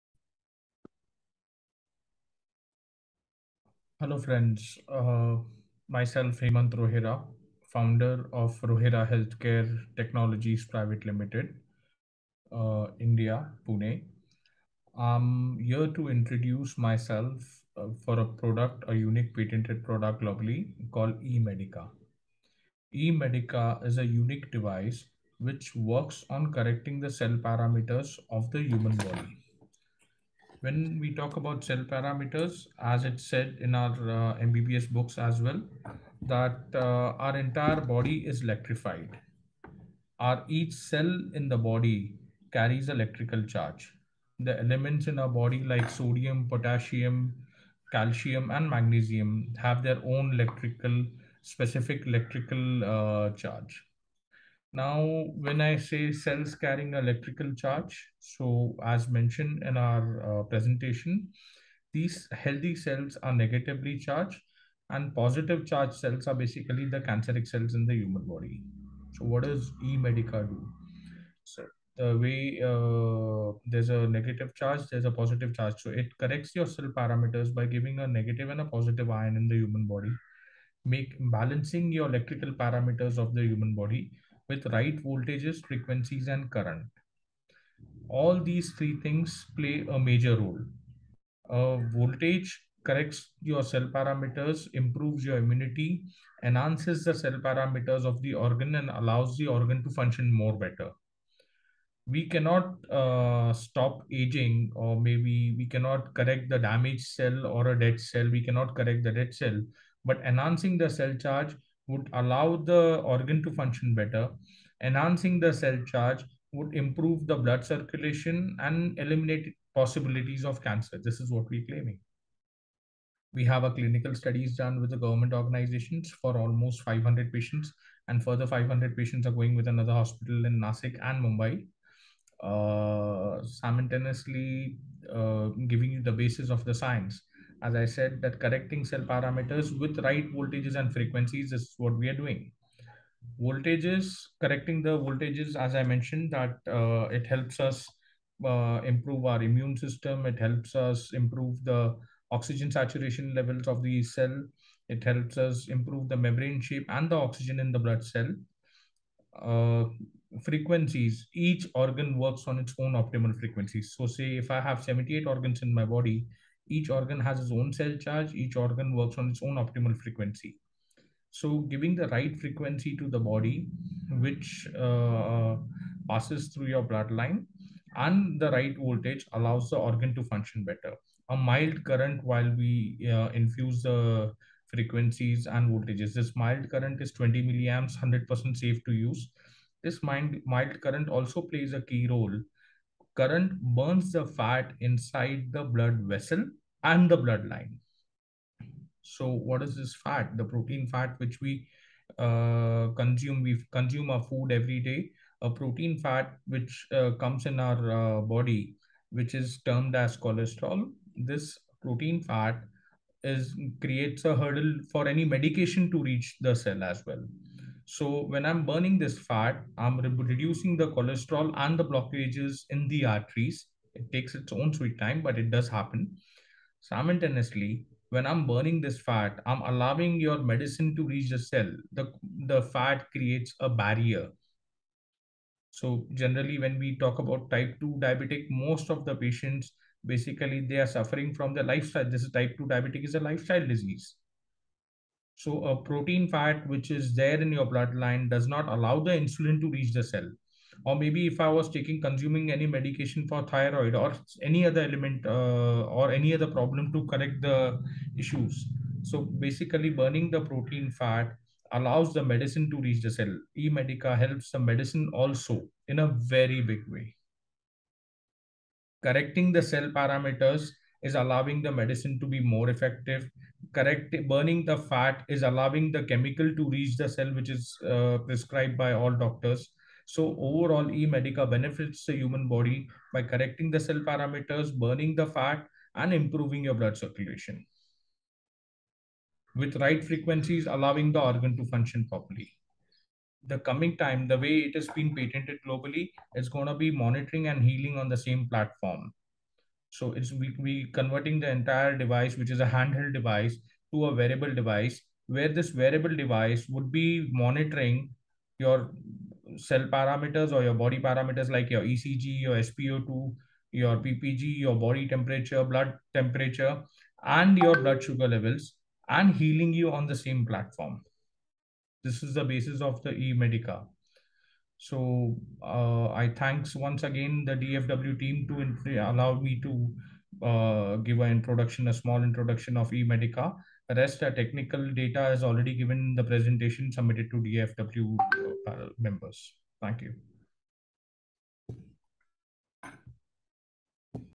Empowering Doctors Worldwide: Introducing eMedica's Revolutionary Healthcare Breakthrough"
Welcome to a groundbreaking revolution in healthcare as we introduce eMedica, a game-changing solution designed to empower doctors worldwide. In this captivating YouTube video, we present eMedica's transformative technology and its potential to reshape the way doctors approach patient care.

Join us as we unveil the future of medicine with eMedica, an electronic medical device that harnesses the power of optimal frequencies and cutting-edge electromagnetic modulation. Discover how eMedica enables doctors to optimize treatment outcomes, revolutionize disease prevention, and enhance patient well-being.

With eMedica's unique ability to generate specific frequency-voltage-current combinations, doctors can now target diseases more effectively than ever before. From preventing chronic conditions to enhancing the body's natural healing processes, eMedica offers an array of revolutionary benefits.@TheDoctors  @doctors-circle @oladoc-FindtheBestDoctors  @canadianassociationofnatur2093 @healthcarereformsdoctorsas6094